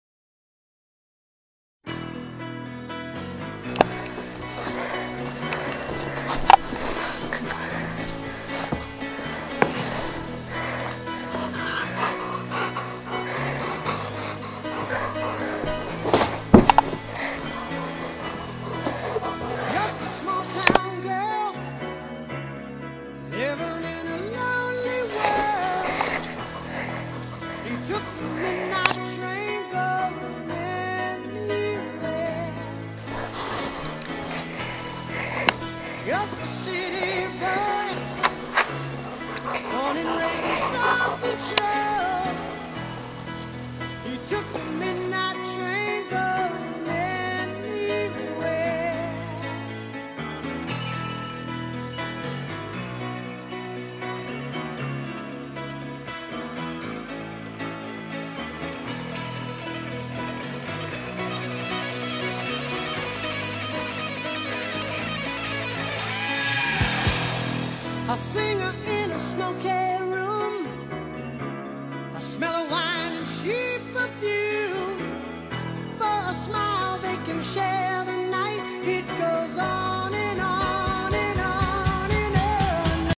Coolest dog ever
my dog wearing a hoody - picture slideshow